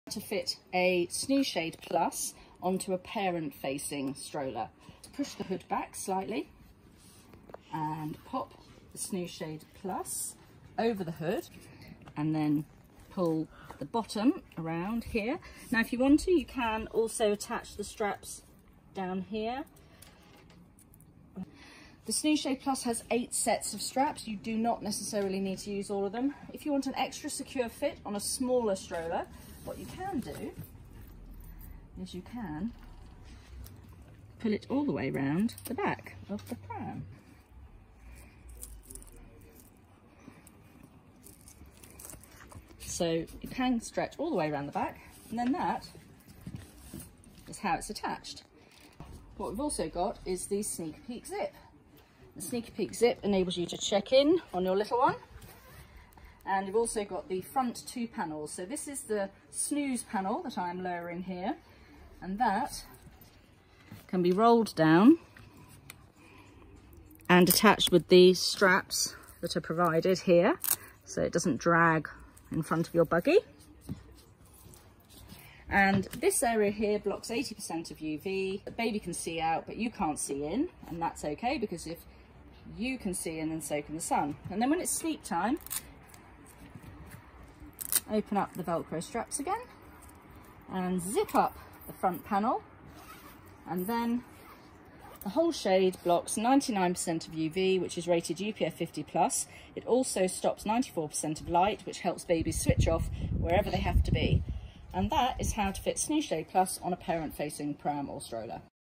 How to use SnoozeShade Plus with a parent-facing stroller
Designed to be used when your baby is around six months old, or once they are sitting upright, SnoozeShade Plus is a much larger version of SnoozeShade Original and boasts additional functionality suited to children who are sitting up and want to see out.

This award-winning sun and sleep shade is made from better-than-breathable mesh fabric that lets air flow freely, safely blocks up to 99% of UV rays (UPF50+) and shuts out light to help keep your little one’s nap routine on track while you’re out and about.

SnoozeShade Plus fits absolutely any single-width pushchair or stroller with a hood and is quick and easy to attach. It will last until your little one is out of the buggy,